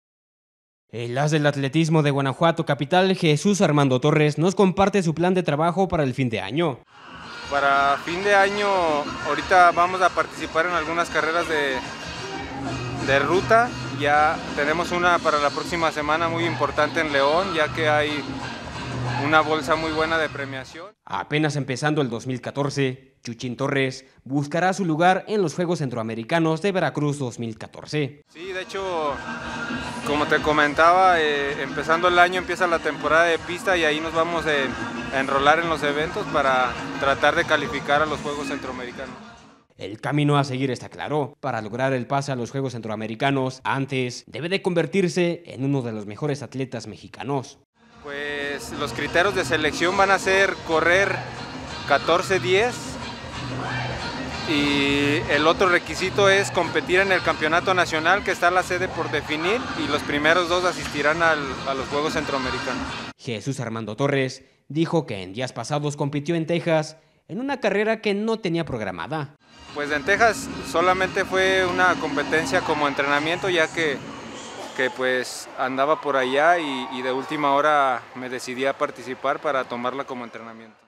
JESÚS ARMANDO TORRES (ATLETA GUANAJUATENSE)
JESÚS ARMANDO TORRES, ATLETA DE GUANAJUATO SE PREPARA PARA LOS JUEGOS CENTRO AMERICANOS DE VERACRUZ 2014